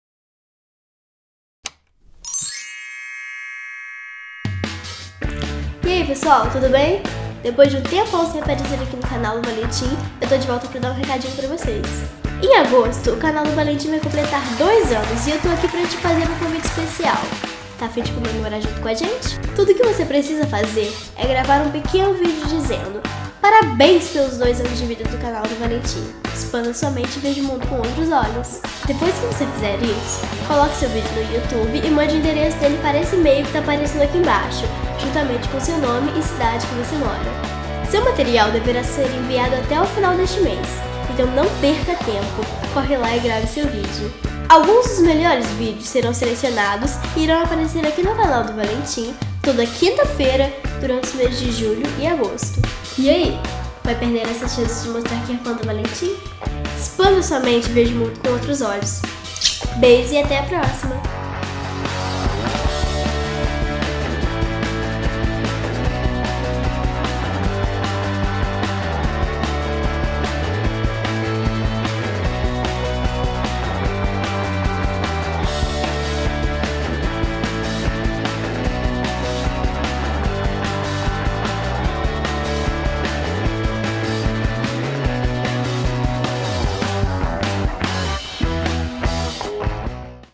Convite Especial. Quer aparecer no Canal do Valentim???
ATENÇÃO!
Dicas para gravar seu vídeo:
* Grave na melhor resolução possível
* Procure um lugar bem iluminado
* Se gravar com celular, grave com ele deitado.
* Grave em um lugar com pouco barulho
*Boa sorte! ;)